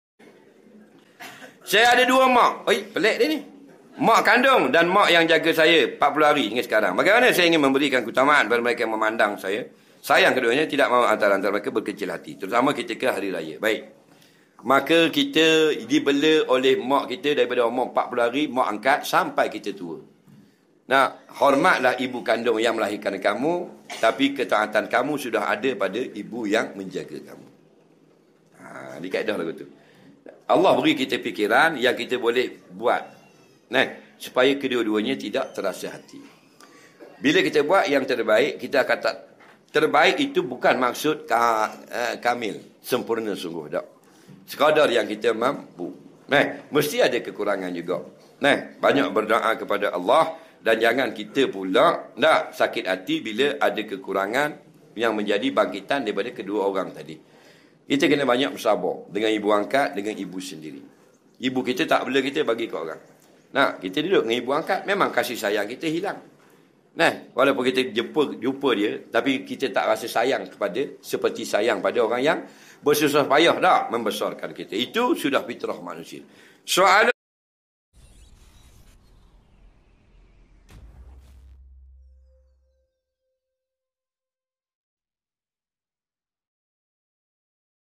Cara Berbakti Kepada Ibu Kandung & Ibu Angkat - Ustaz Azhar Idrus Official
Kuliyah Dhuha Jemputan Ustaz Azhar Idrus Official

⏰ 09.00 am
🕌 Mahad Tahfiz Az-Zein Tmn Kiambang Indah Sebang,  Senawang Negeri Sembilan